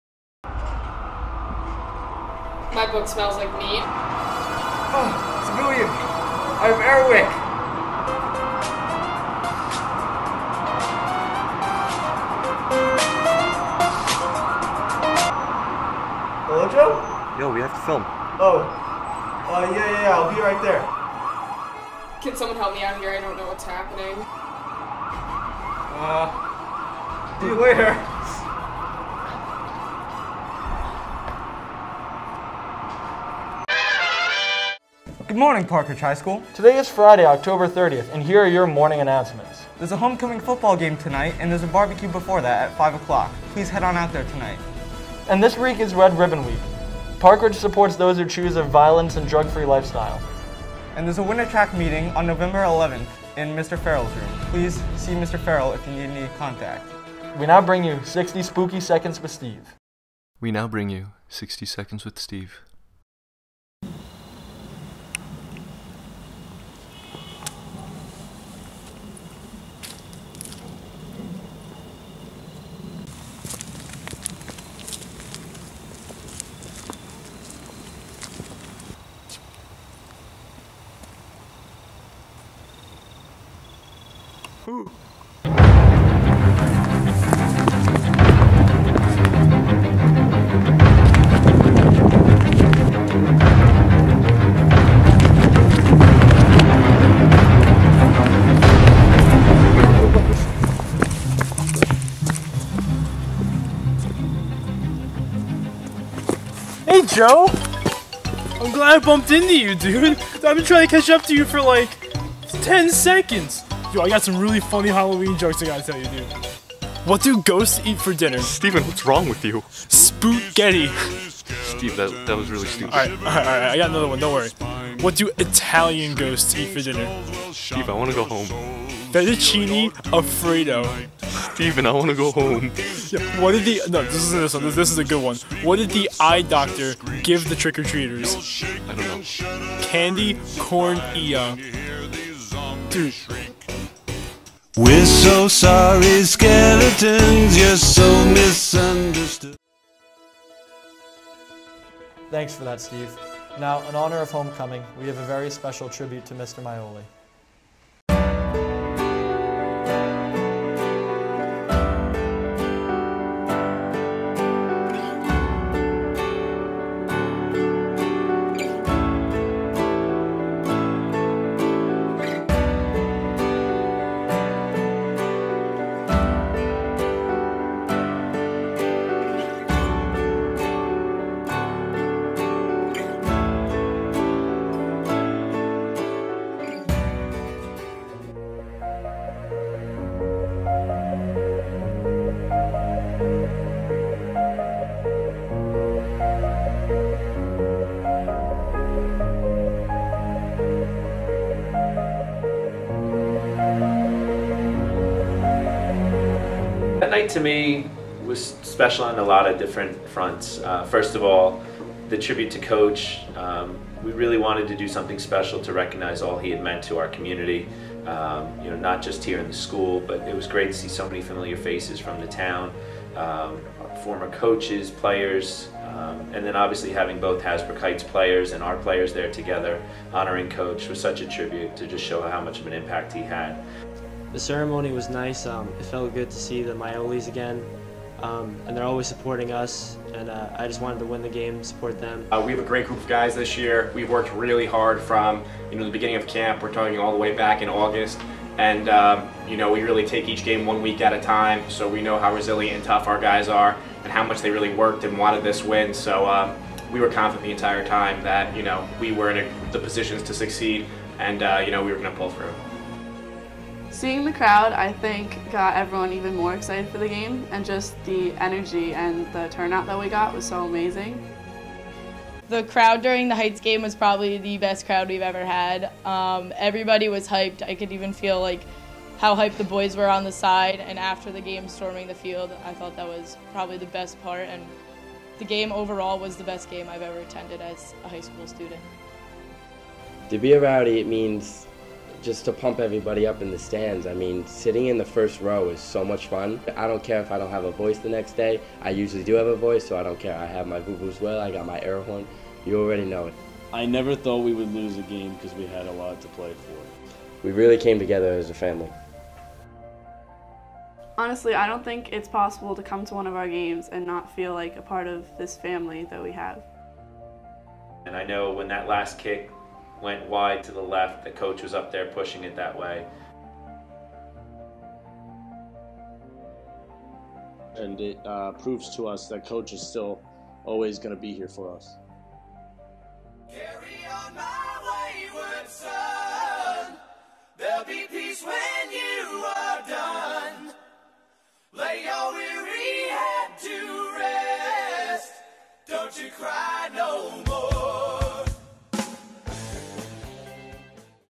10.30.15 Owlwitness News
This is the fourth installment of Owlwitness News of the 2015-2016 School Year.

Coordinator: Tina Bacolas
Editor/Producer: Joseph Kim and Kevin Yu
Anchors: Spiderman and Gorilla
Sixty Seconds With Steve: Steven Rizzie and Joseph Kim

Have a Spooky Halloween!